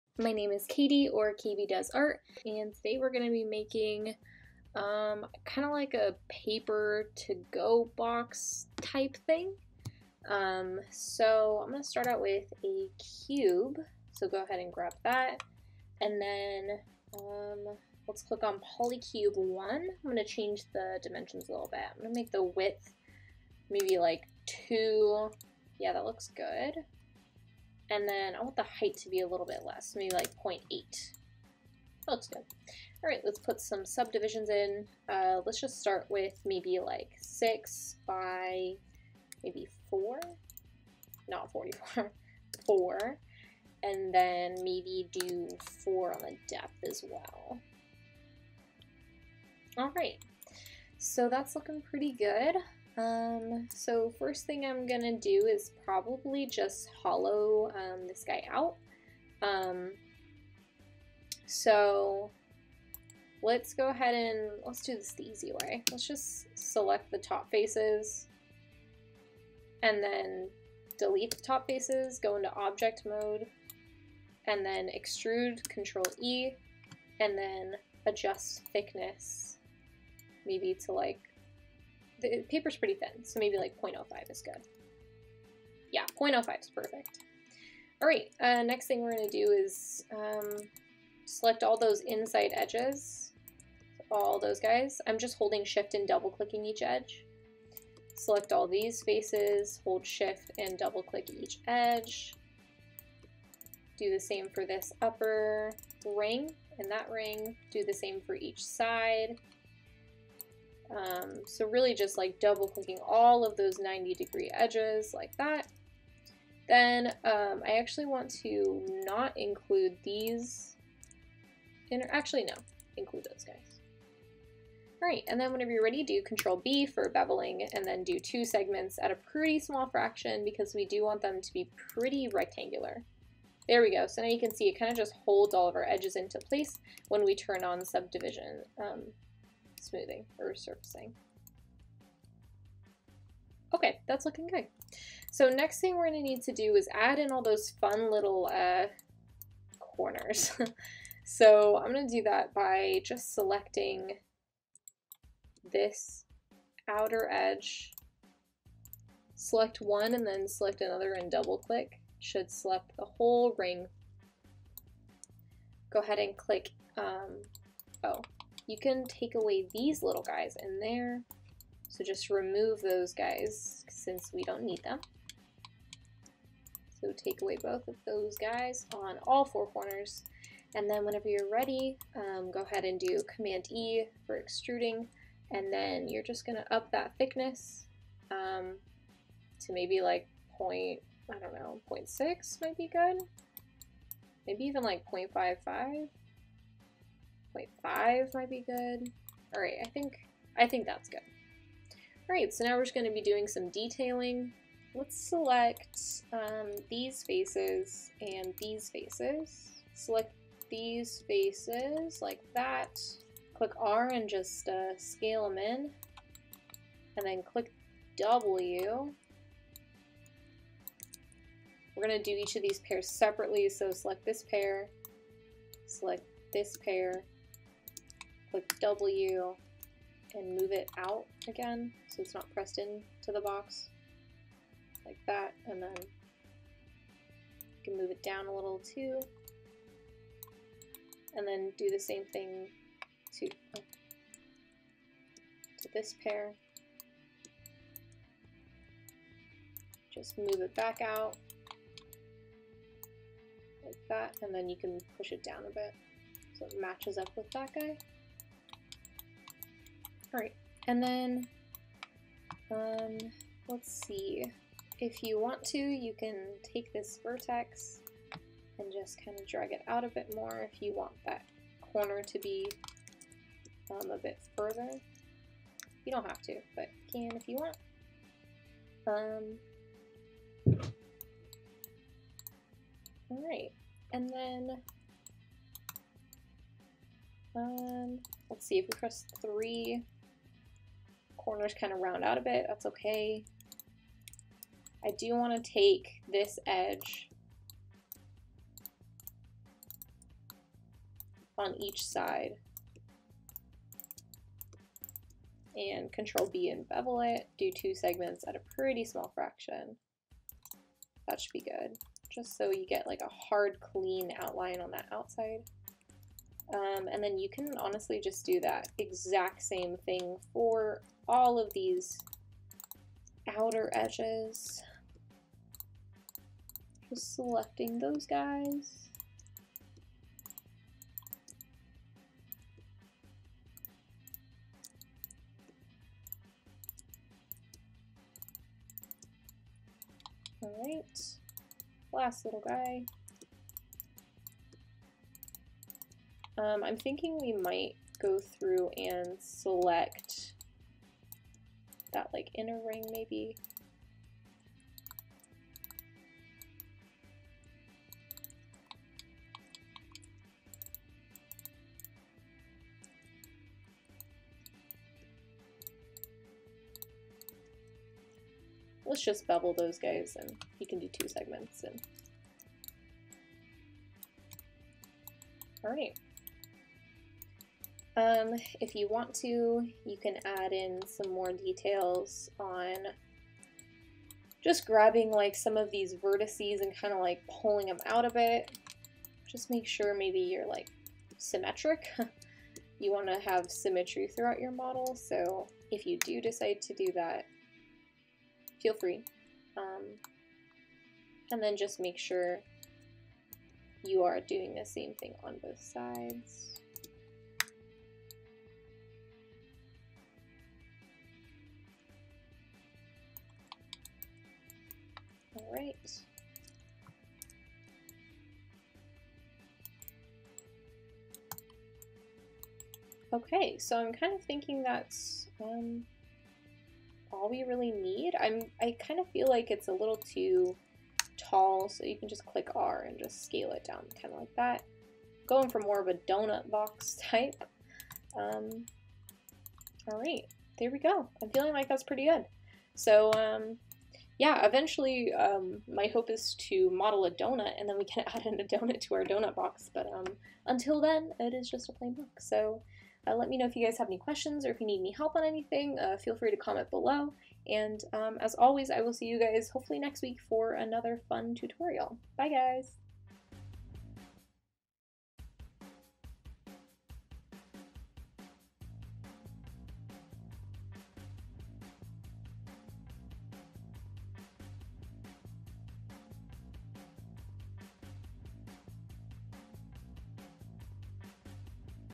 Modeling a Donut Box - Maya Tutorial
In this tutorial, I walk you through modeling a donut box in Maya! I hope you guys enjoy!

NEW TUTORIAL PREMIERES EVERY MONDAY AT 5 PM!

❤ I can't do this without you guys, consider buying me a coffee!